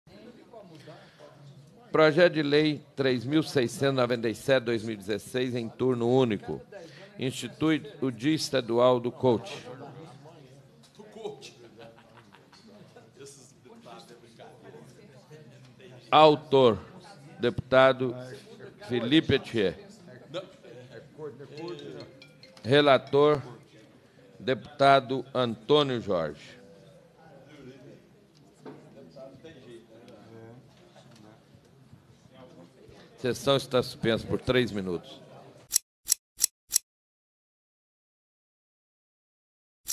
Deputado de Minas zomba de projeto de lei que ele mesmo apresentou
"Esses deputados... É brincadeira! Não têm jeito", comentou Felipe Attiê (PTB)